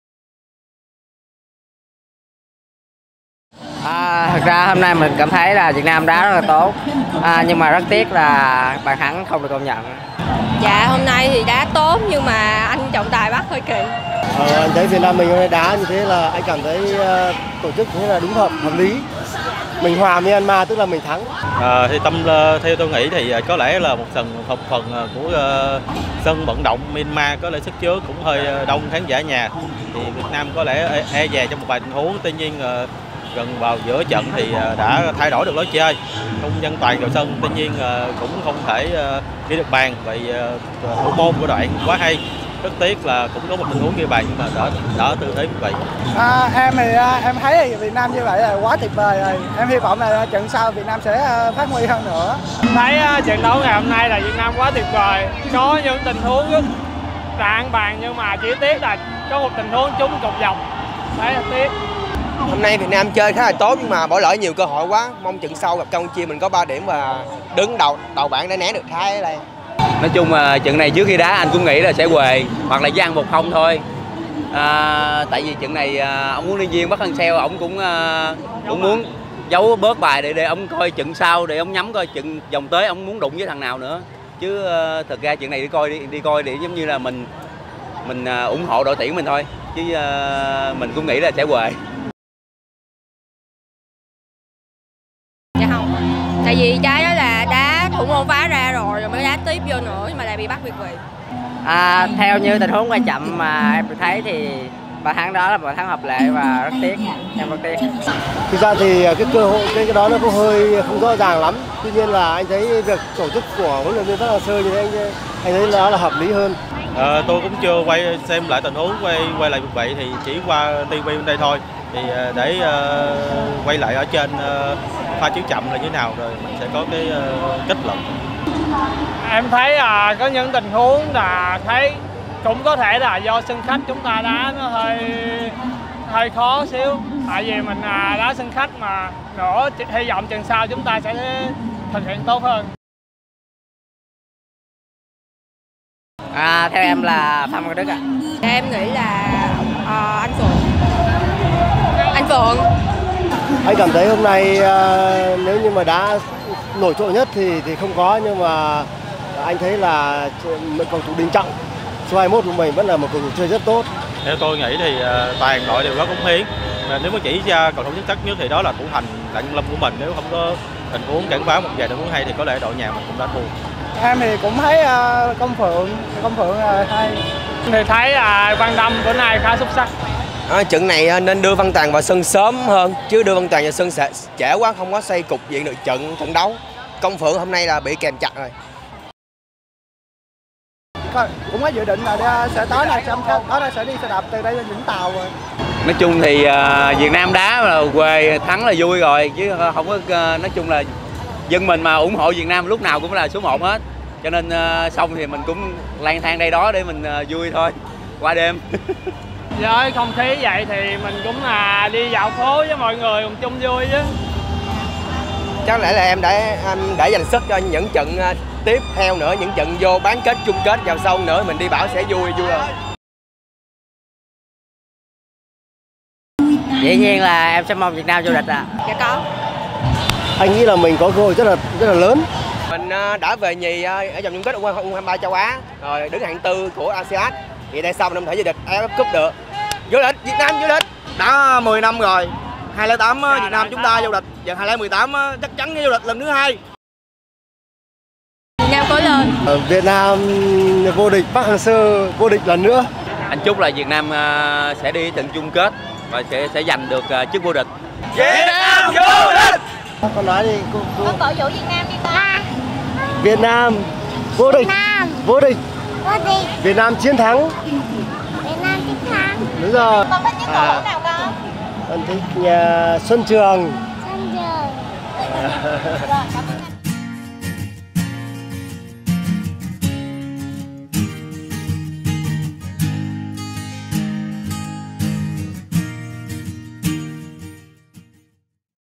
CĐV nhí thích Xuân Trường, người hâm mộ tin tưởng chức vô địch AFF Cup năm nay
AFF Cup Vietnam - Myanmar: CĐV nói gì về bàn thắng Văn Toàn bị thổi việt vị và Ai là người xuất sắc nhất trận?